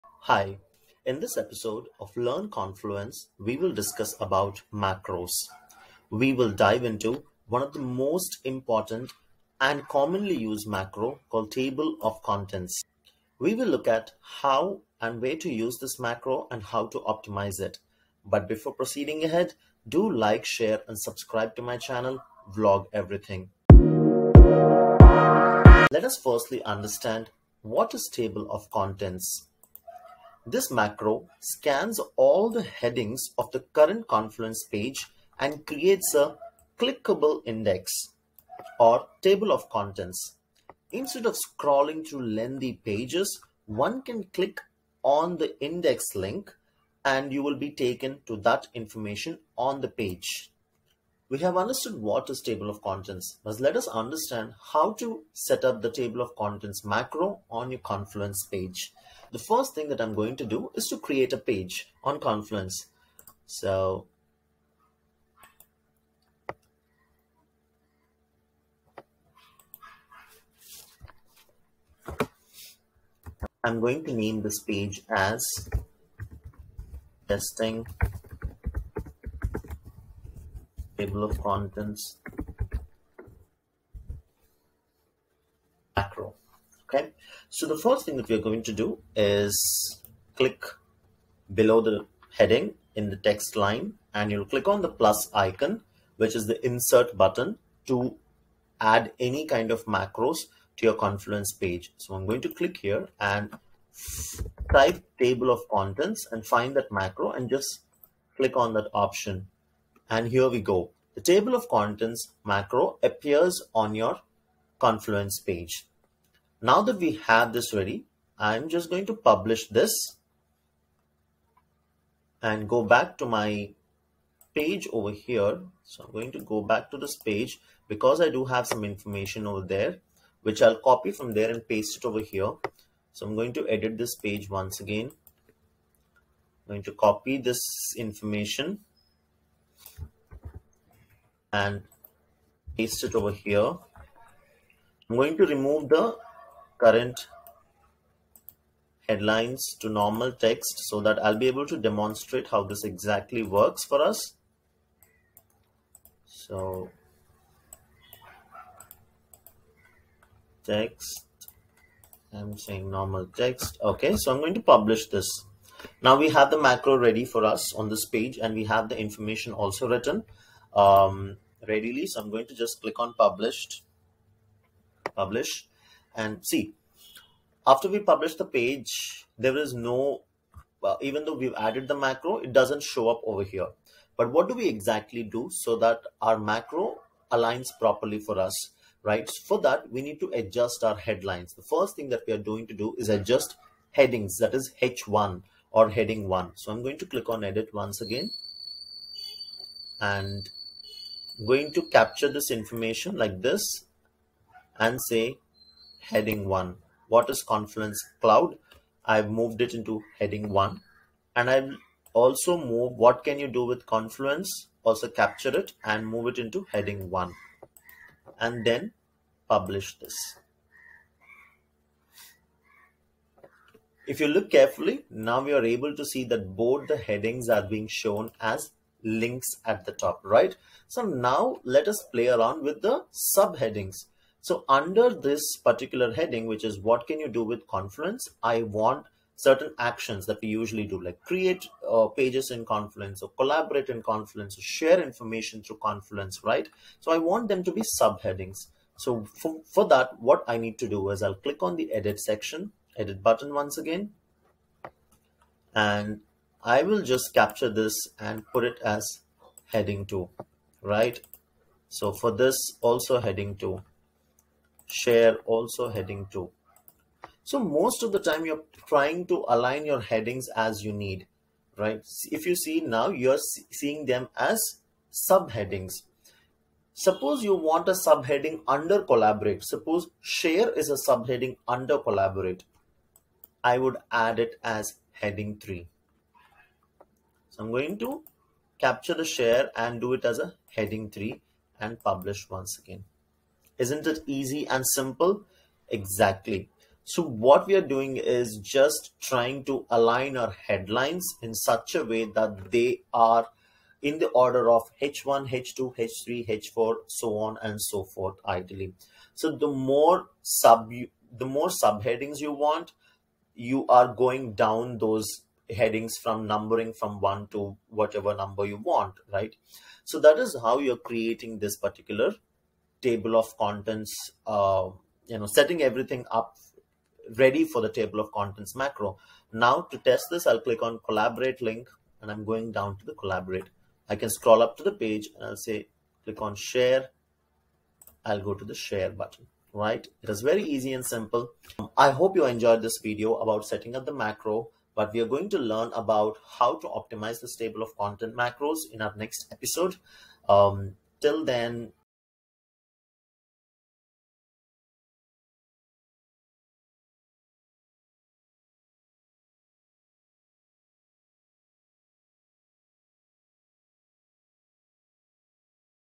How to add Table of Contents Macro in Confluence | Confluence Tutorial for Beginners | Macros
In this Confluence tutorial, we will learn how to add the Table of Contents Macro in Confluence. The table of Contents macro in Confluence creates a table of contents based on page headings.

⌚ Time Stamps:
00:00 Intro
00:26 What is Table of Contents Macro?
01:28 Adding Table of Contents Macro
03:11 Adjusting Headlines (H1, H2, H3..) for the Macro to work
05:30 Testing Table of Contents Macro